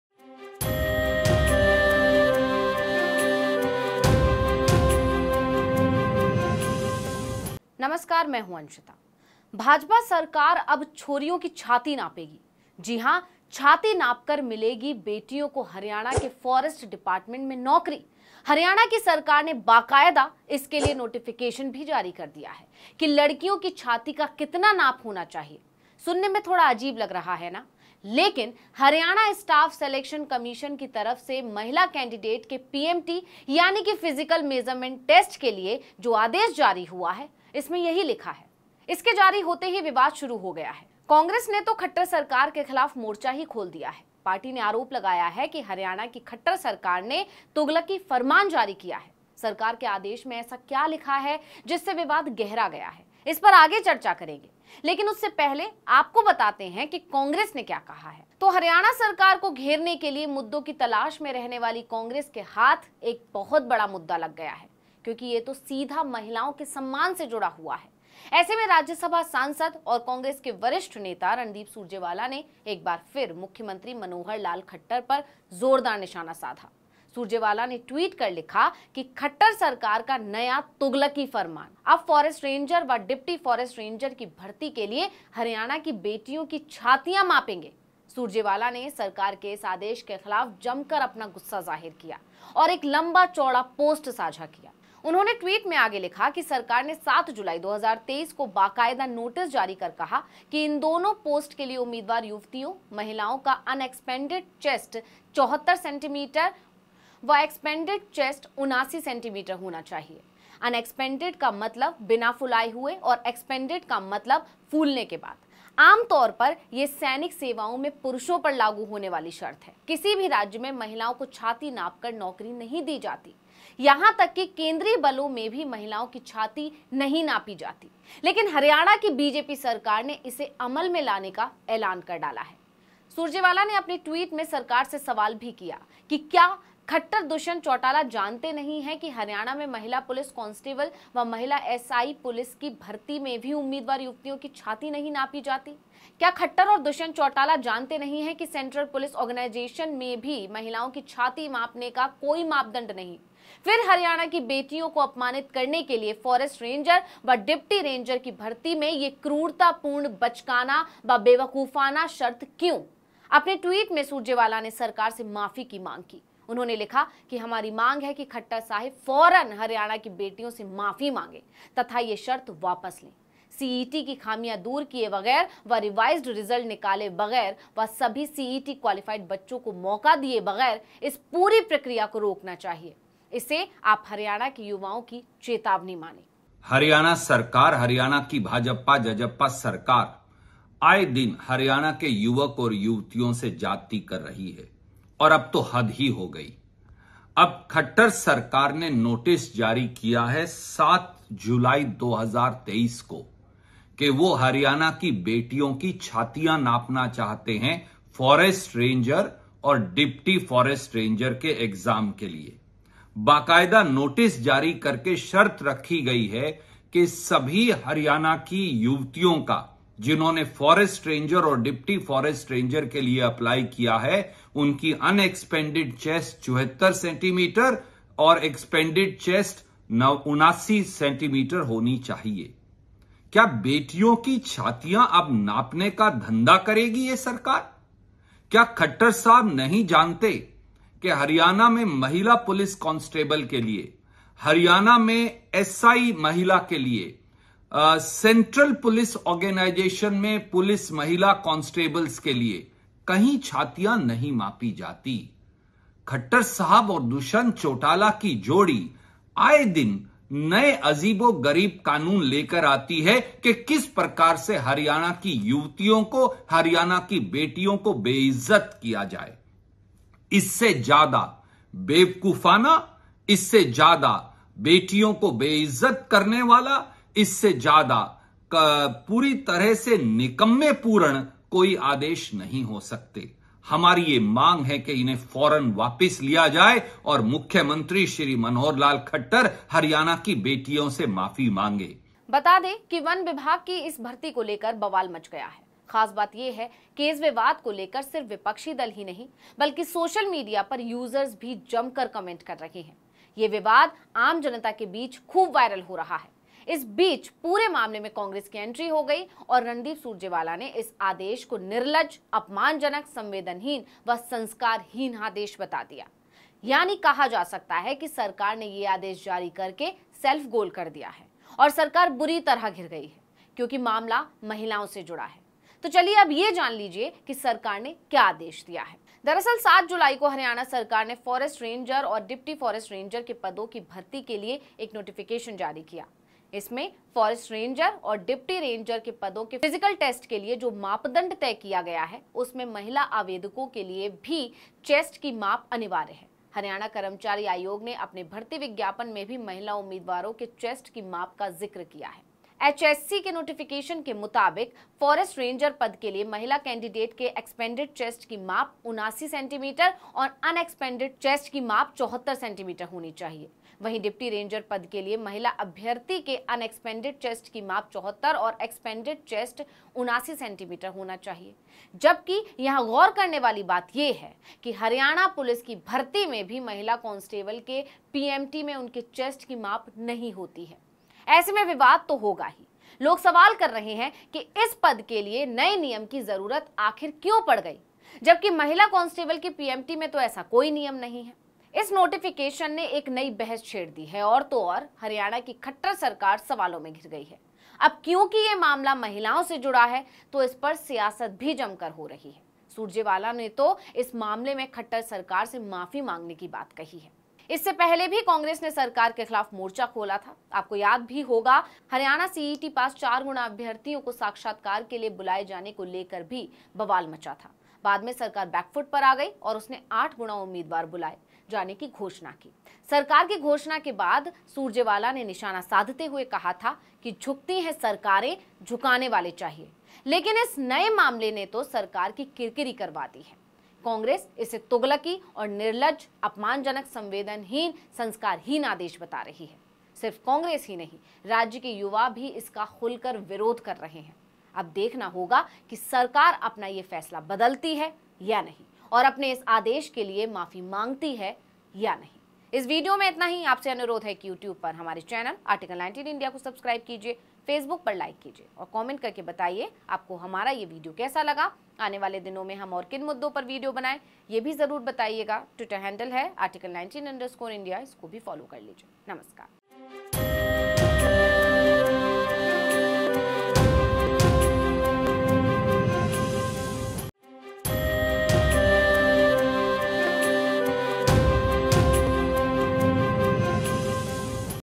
भाजपा की सोच पर उठे सवाल, क्या यही है आपका नारी सम्मान
पूरे देश में किसी भी सेवा में लड़कियों की छाती नहीं नापी जाती। भाजपा ने अपन राज्य से इसकी शुरुआत की है। सोशल मीडिया पर जमकर खरी-खोटी सुना रहे हैं लोग।

फेसबुक @article19india,
ट्विटर @Article19_India